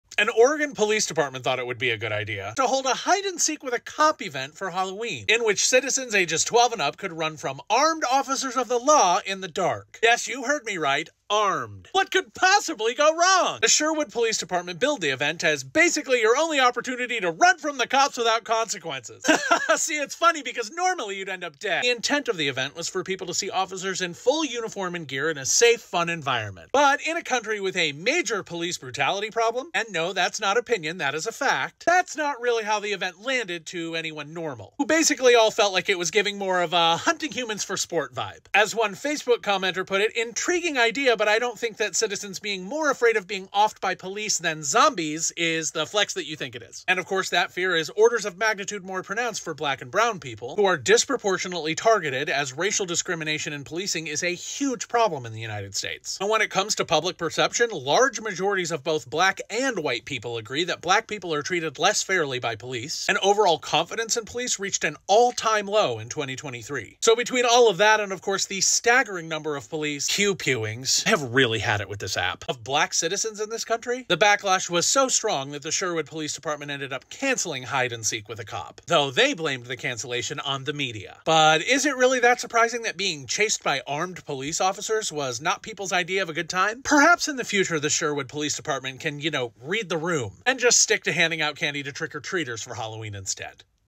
Oregon police cancel their 'Hide & Seek With A Cop' Event After Community Backlash
The event was marketed as 'your only chance to run from the cops without consequences.' But it turns out, the community wasn't sure that this was exactly their idea of a good time.